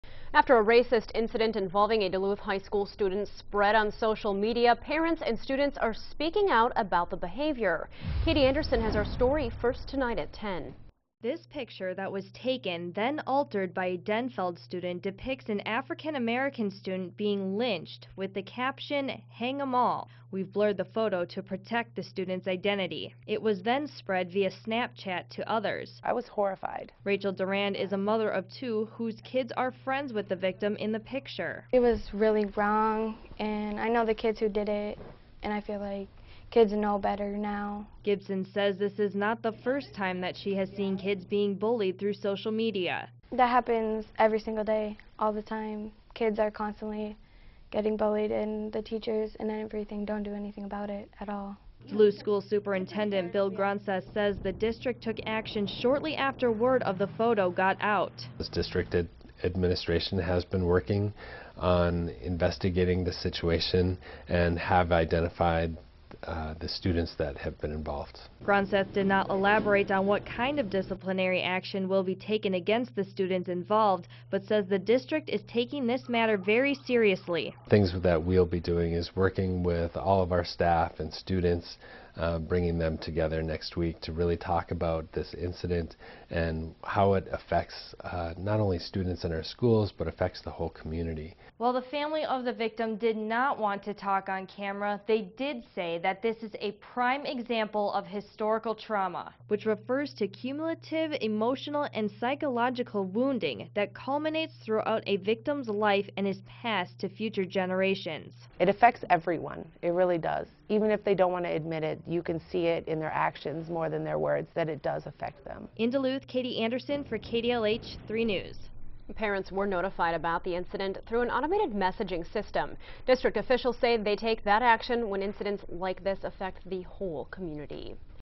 Racist photo circulating within Duluth school, rope around student’s neck, captioned "hang' em al
Racist photo circulating within Duluth school, rope around student’s neck, captioned "hang' em all"